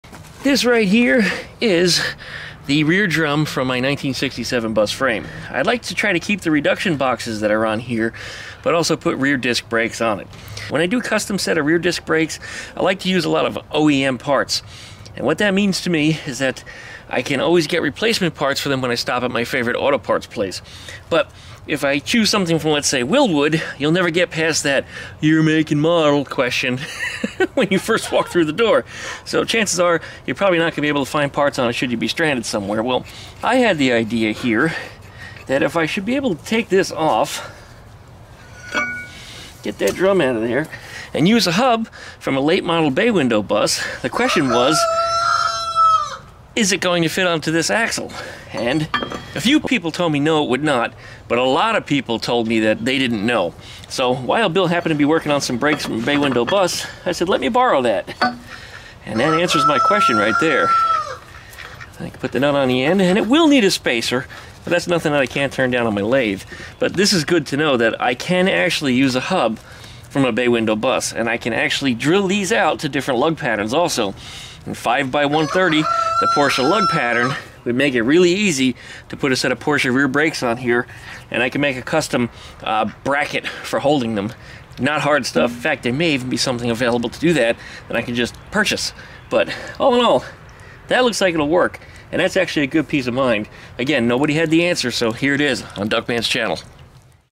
🔧💥Bay Bus Hubs on Split Reduction Boxes?
It's no big secret that I like 4 wheel disc brakes. for a multitude of reasons. But options are very limited (and expensive) should I plan to use a set of reduction boxes on this Bus. Well,the hardest part is to find a hub that fits on the axle. Everything else is simple fabrication work after which. So will a BayWindow Bus hub fit on a Split big axle reduction box?

⚫ Stuff Seen in Video ⚫
Hazel & Creme Gift Box
Drum Puller
Torque Multiplier
Portable Compressor/Jumper
Phosphoric Acid

⚫ More Popular DuckmanCycles VW Videos ⚫